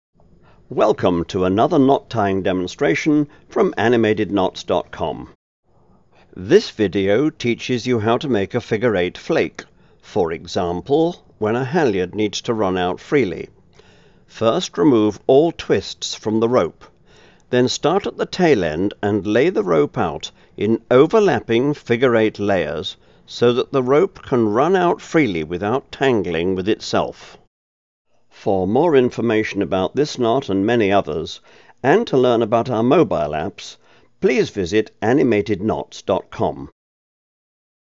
Figure 8 Flake | How to Coil (Flake) a Rope
Figure 8 Flake - Learn how to lay out rope  in a simple step-by-step video.
By AnimatedKnots.com - the world's #1 knot site.